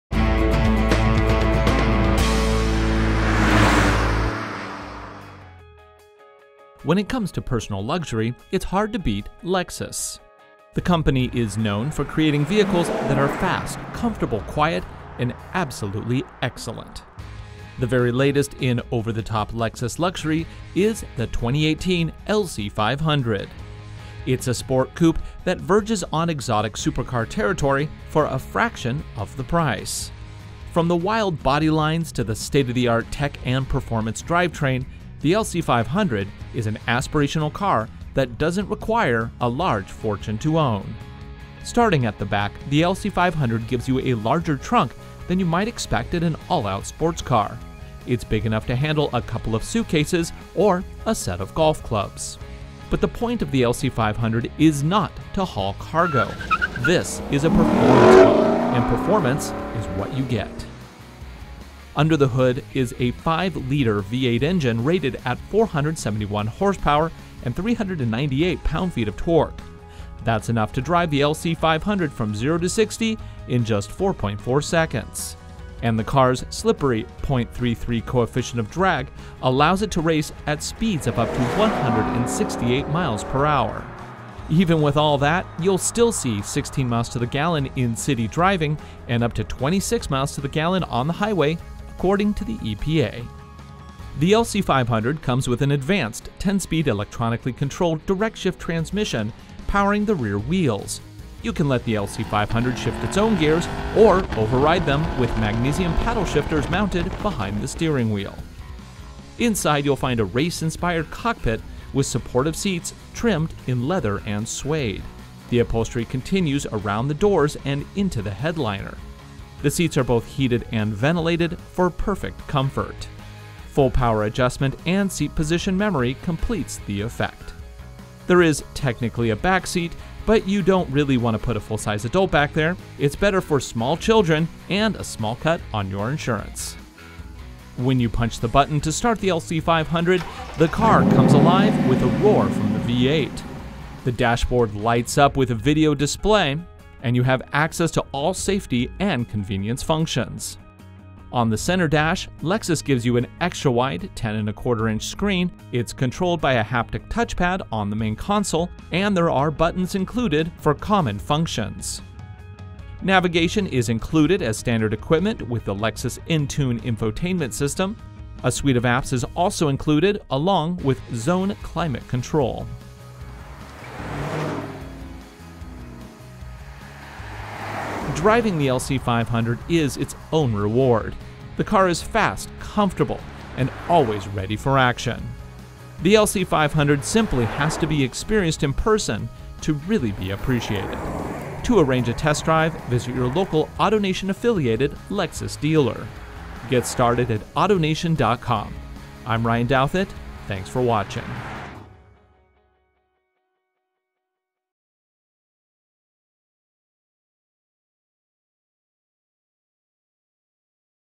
2018 Lexus LC 500 Review - AutoNation
The 2018 Lexus LC 500 Coupe is the latest installment in Lexus' long line of superbly comfortable and luxurious vehicles. The 2018 Lexus LC 500 verges on exotic supercar while keeping its price tag accommodating. Does it deliver? Let's find out.

The 2018 Lexus LC 500 Coupe is powered by a 5-liter V8 engine that puts out 471-horsepower and 398-lbft of torque. This engine launches you from 0 to 60-mph in just 4.4 seconds, and is paired with a state-of-the-art drivetrain.

The advanced 10-speed electronically-controlled direct shift transmission feeds power to the rear wheels and shifts on its own. Want to take driving into your own hands? Paddle shifters are mounted behind the steering wheel.

With a functional trunk and decent gas mileage, the 2018 Lexus LC 500 Coupe is a dreamy multi-faceted sports car.